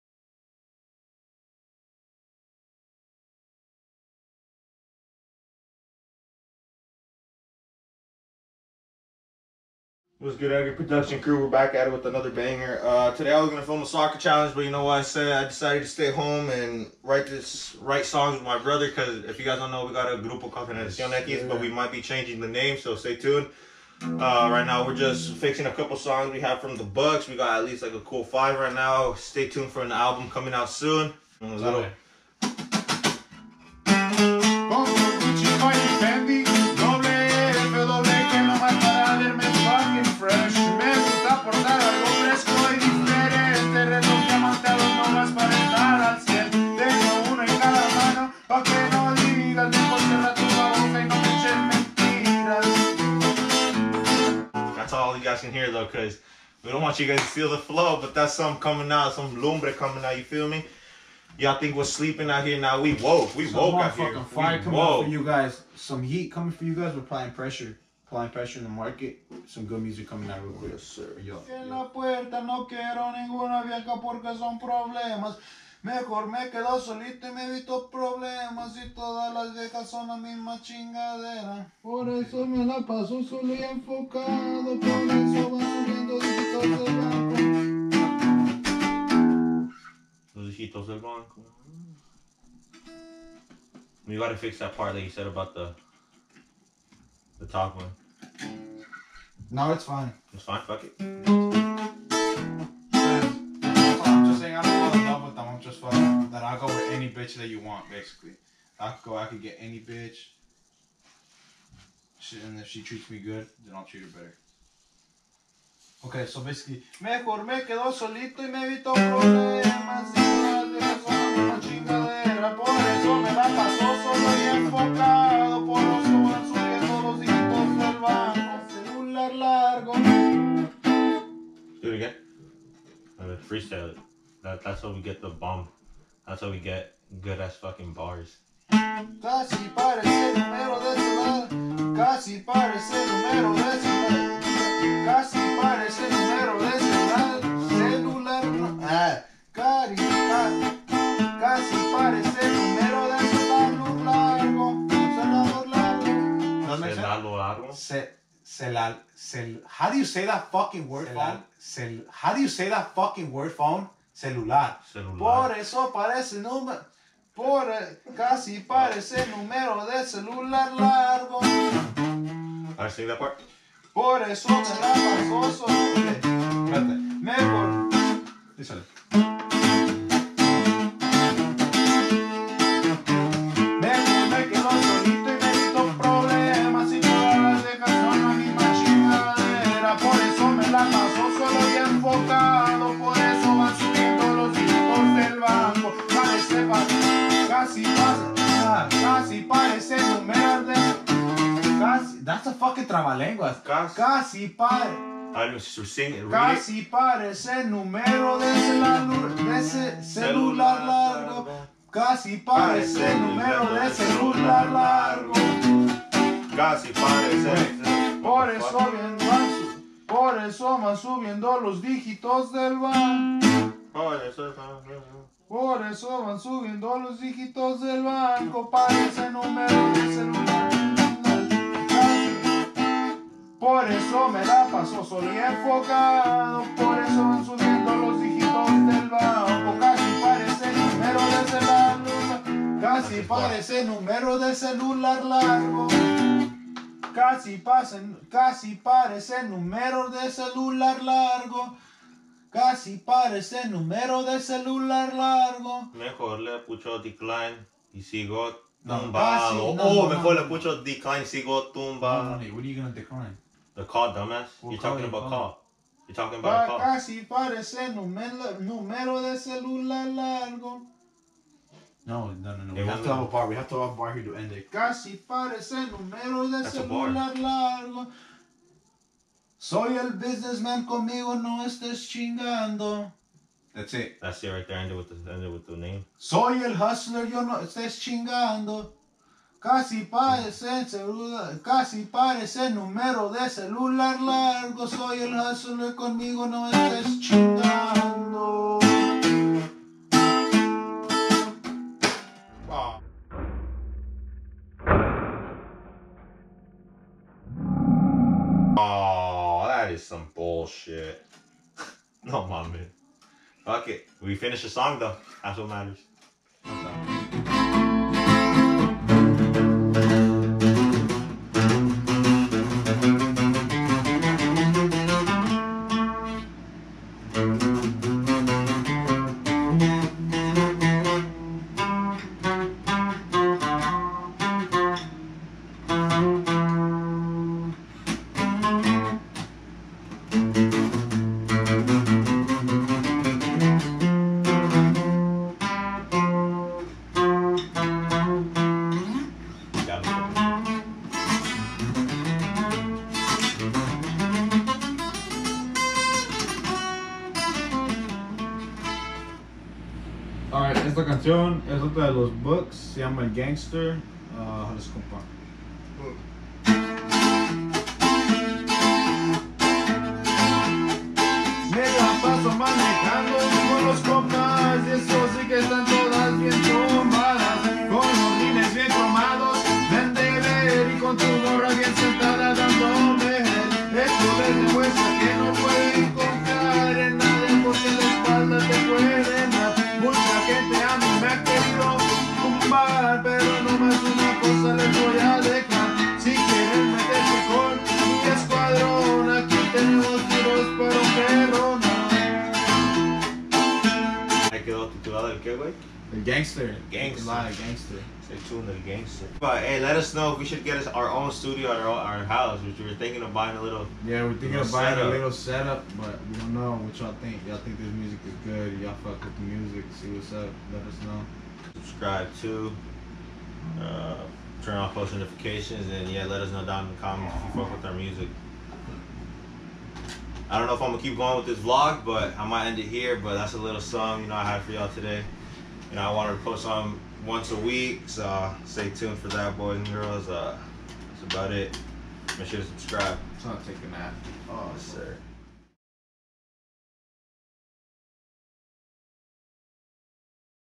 GeneraciónX Vlog#1
We basically show you guys the process we go through to write our own music!